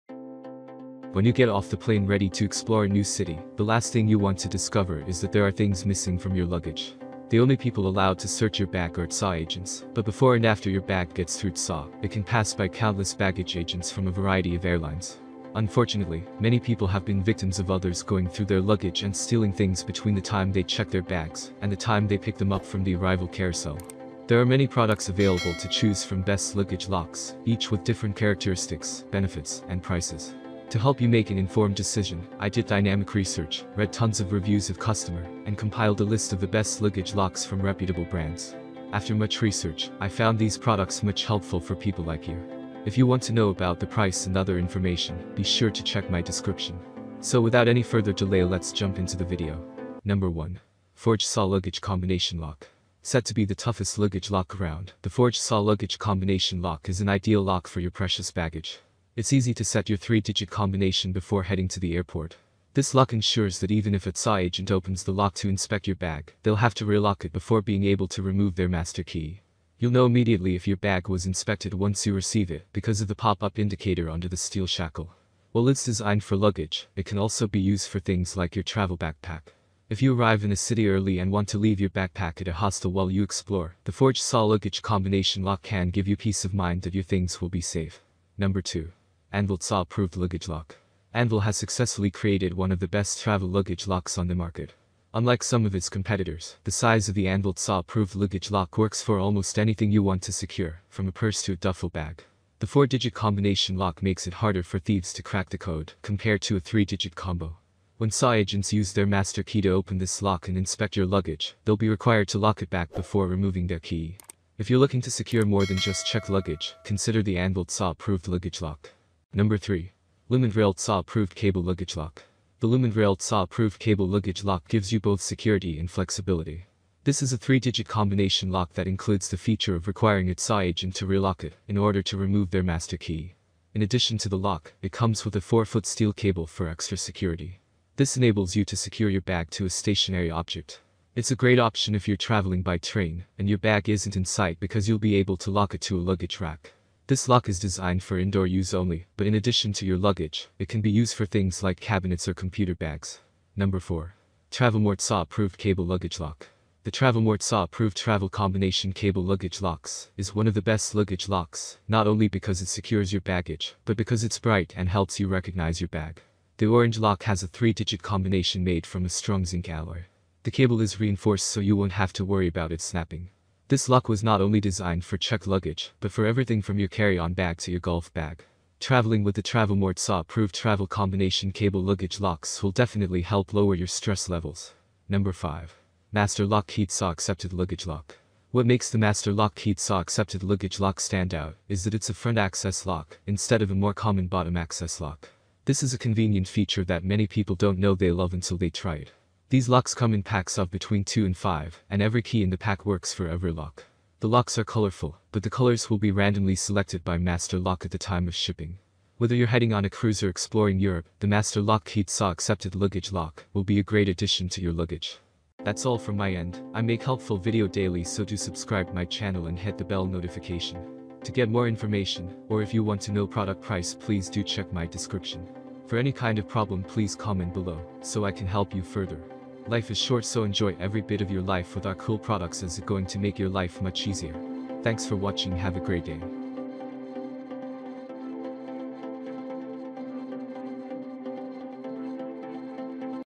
Top 5 Best Luggage Lock On Amazon 2023
👨🏻‍🔧✔️Best Luggage Lock 🔥 💥
📌Product Link📌
✅ My Picks ✅1. Forge TSA Luggage Combination Lock
✅ Best Value ✅2.  Anvil TSA Approved Luggage Lock
✅ For Beginners ✅3. Lumintrail TSA Approved Cable Luggage Lock
✅ Premium ✅4.  TravelMore TSA Approved Cable Luggage Lock
✅ Trending ✅5. Master Lock Keyed TSA Accepted Luggage Lock